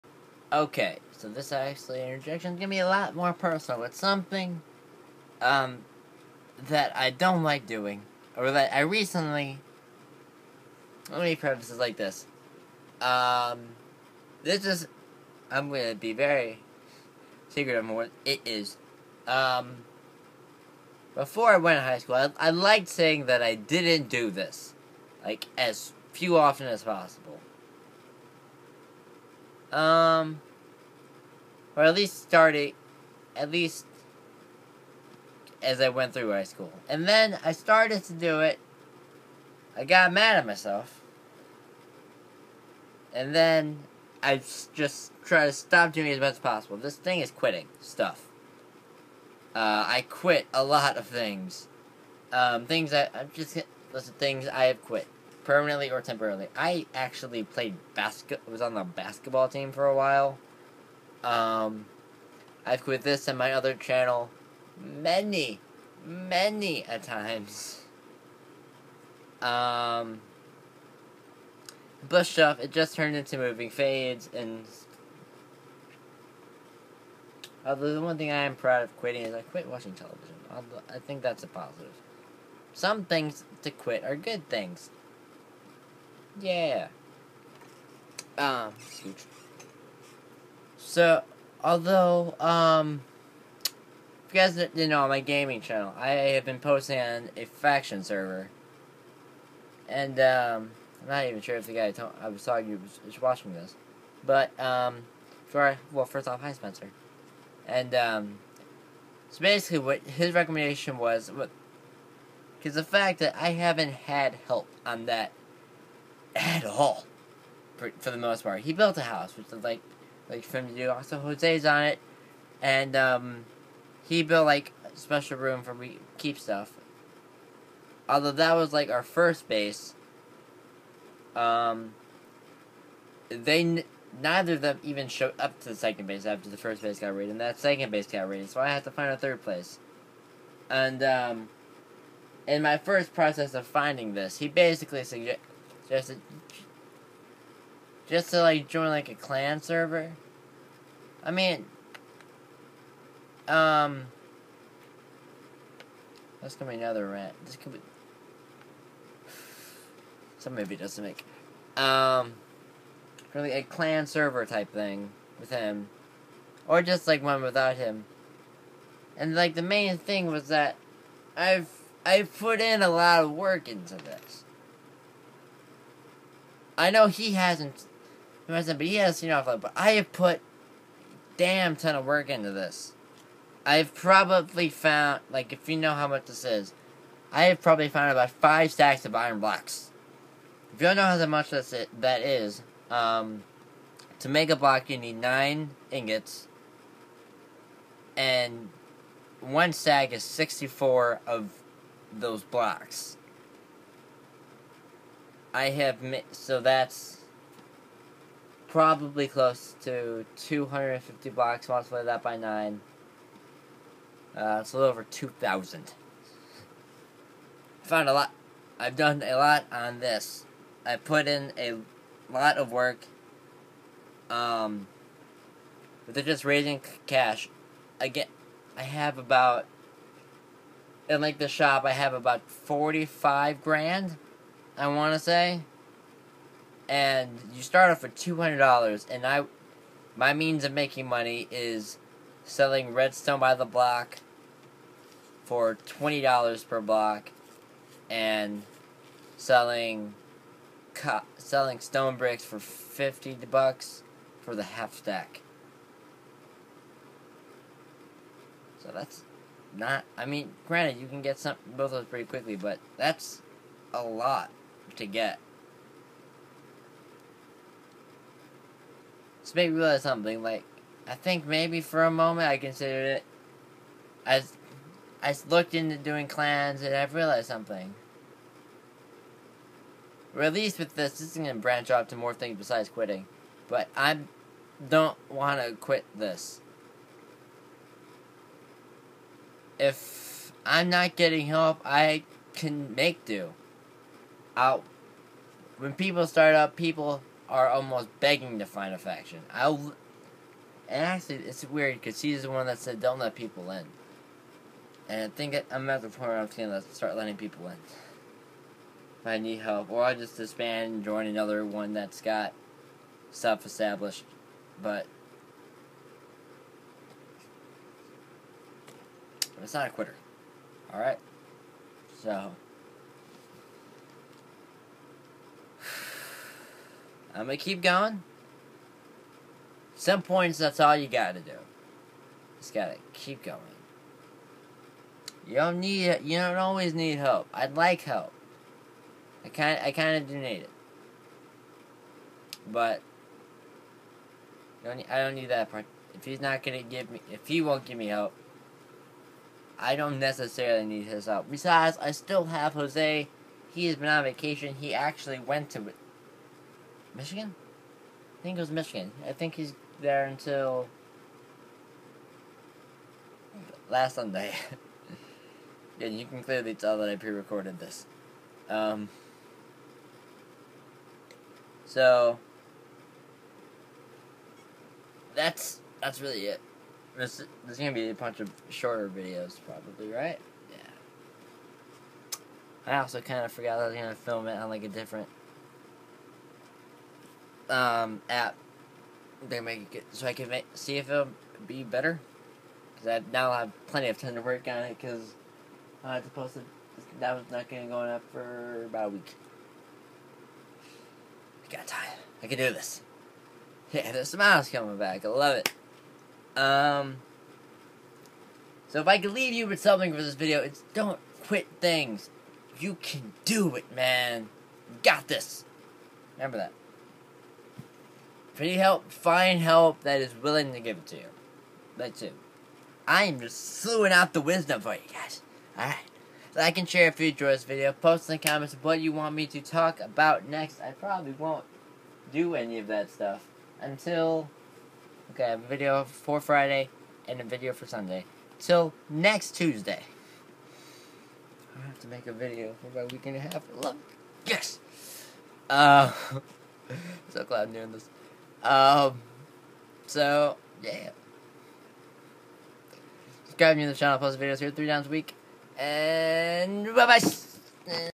Quitting and Persistence
Hey guys, sorry if this is later for you than usual, I genuinely forgot today was Tuesday and I'm very glad I prerecorded. I believe I talked about quitting in this video, and quick life lesson, rarely do it. Sometimes quitting is good (ie smoking or terrible relationships with people), but it's not good in most situations. Comment what you want me to talk about next. Bybyce!!!